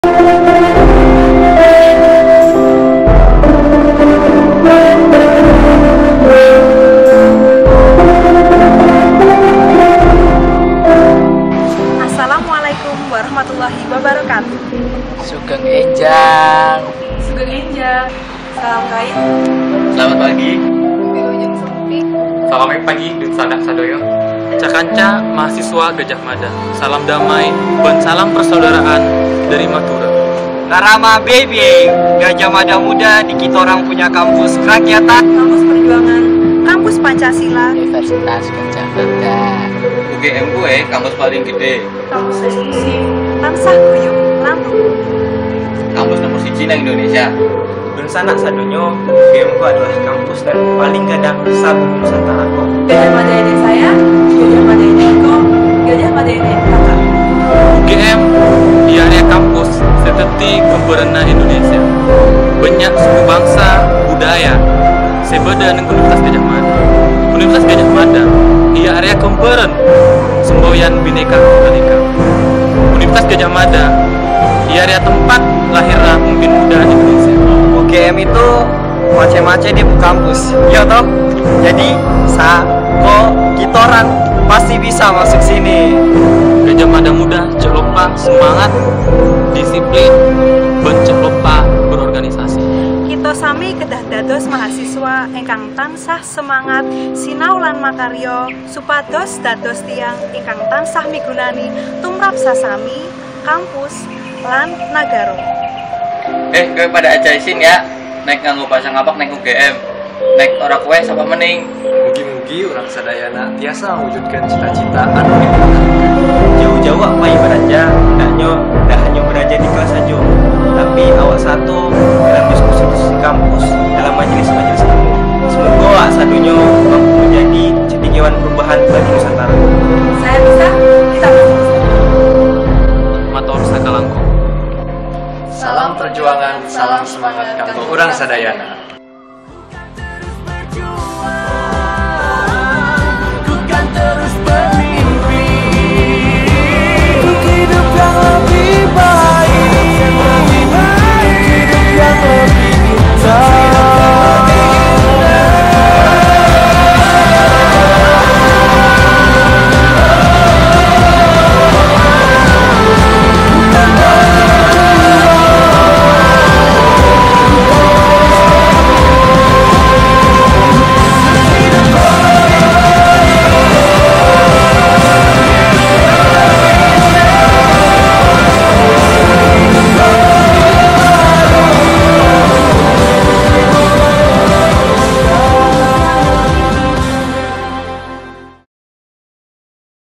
[PPSMB PALAPA UGM] Salam Bhinneka untuk Gadjah Mada Muda 2014
Selamat datang Gadjah Mada Muda 2014 di  Universitas Gadjah Mada.dari beragam latar belakang, suku, budaya, dan bahasa. Selamat menuntut ilmu di kampus ini.

Video Created By : PDD Palapa UGM 2014